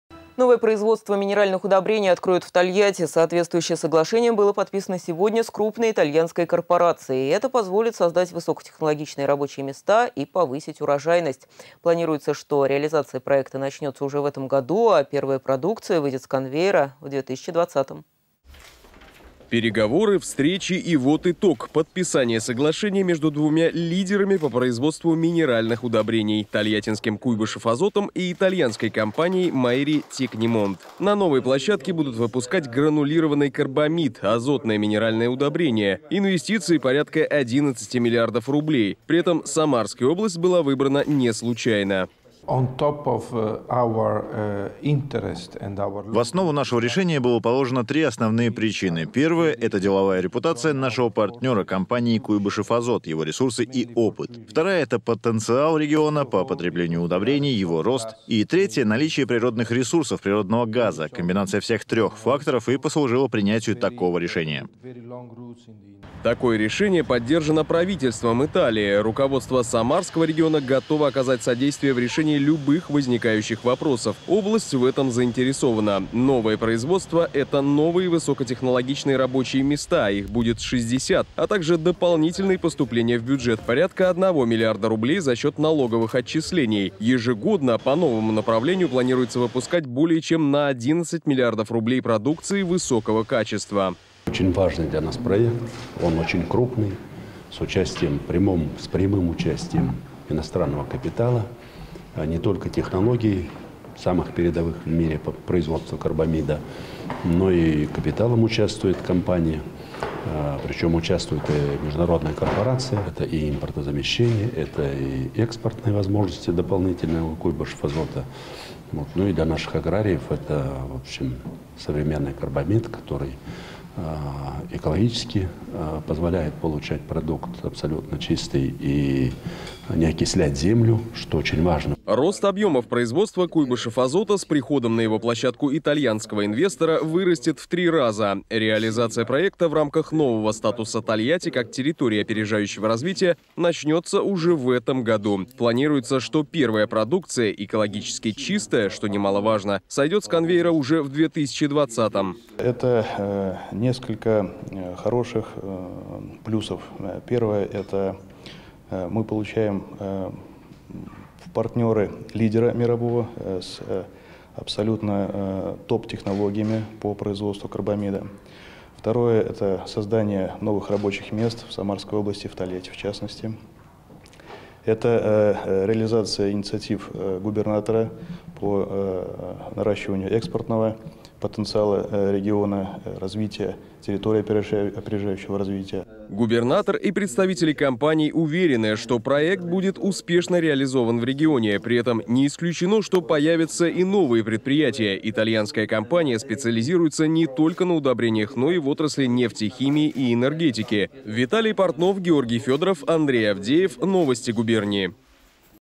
Итальянцы будут делать удобрения в Тольятти
Новое производство минеральных удобрений откроют в Тольятти. Соответствующее соглашение было подписано сегодня с крупной итальянской корпорацией. Это позволит создать высокотехнологичные рабочие места и повысить количество урожая. Планируется, что реализация проекта начнется уже в этом году, а первая продукция с конвейера выйдет в 2020.